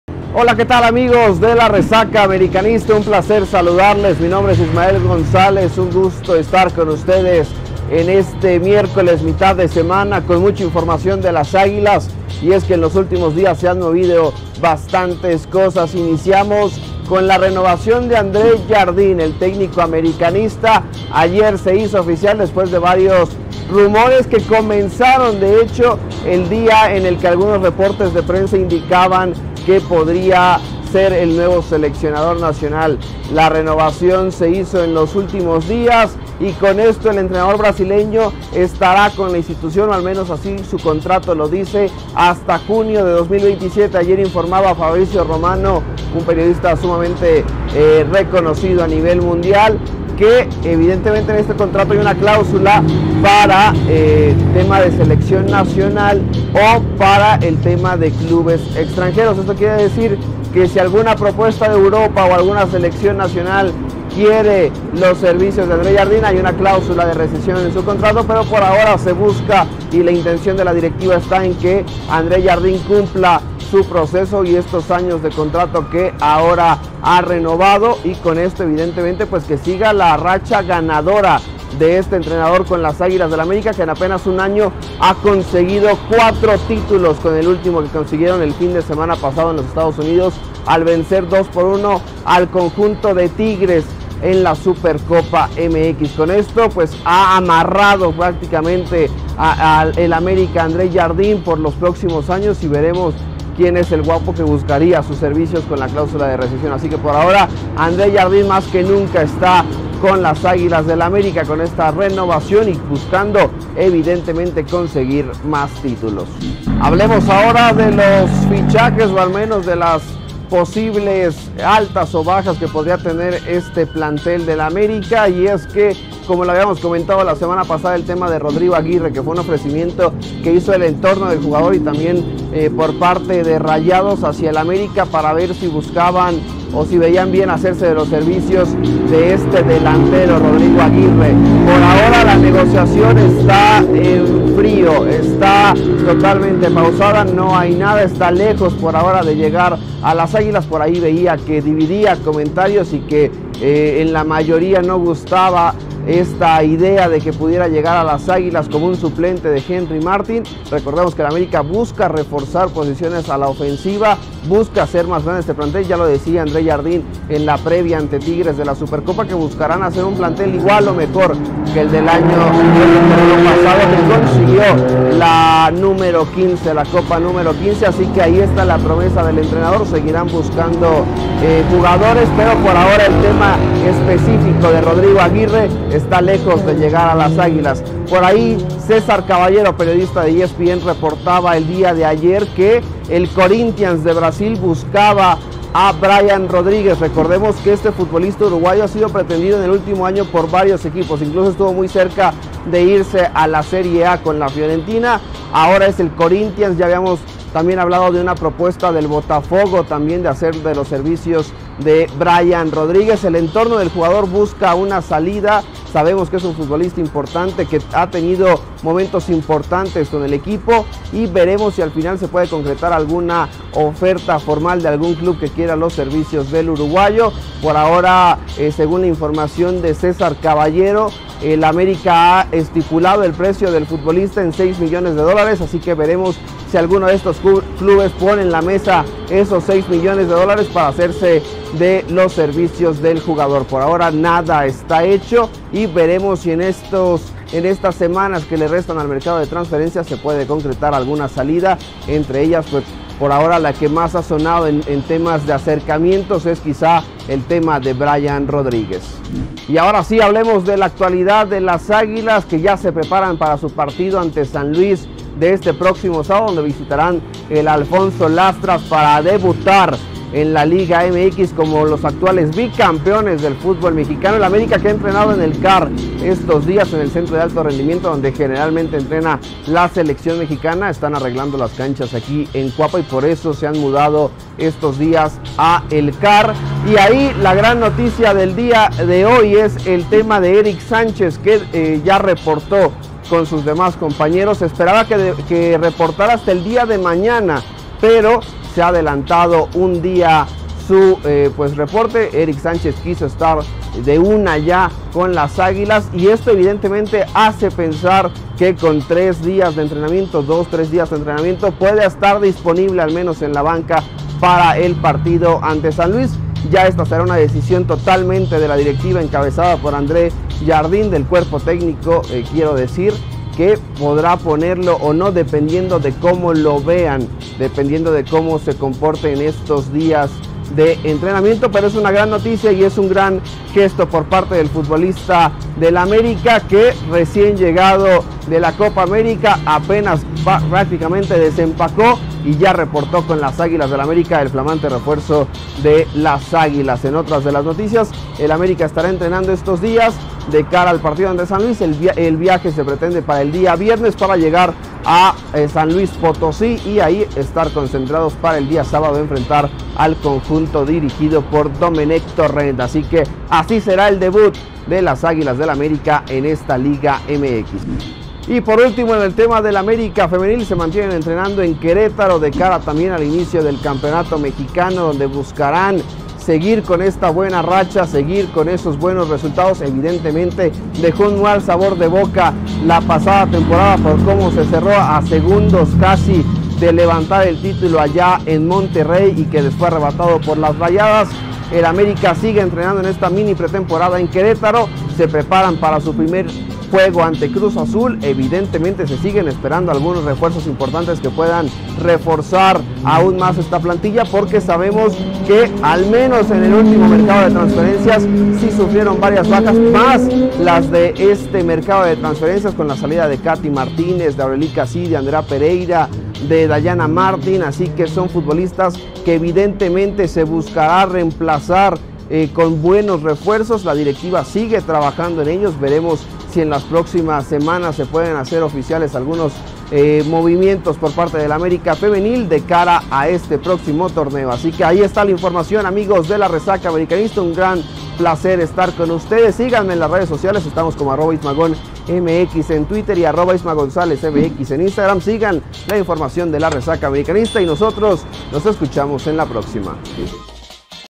¡¡ÚLTIMOS RUMORES DE REFUERZOS Y FICHAJES DEL AMÉRICA APERTURA 2024!! | Noticias América Hoy
🚨¡¡Últimas Noticias América Hoy!!💛💙

- ¿BRIAN RODRÍGUEZ A BRAZIL? 💣✅
- ¿SE CAE LO DE RODRIGO AGUIRRE? 🦅🇺🇾
- YA ESTÁ AQUÍ ERICK SÁNCHEZ 🇲🇽😱

Aquí te lo contamos 👇🏼👇🏼

Algunas fotografías utilizadas en esta producción son propiedad de la Agencia Fotográfica Mexsport.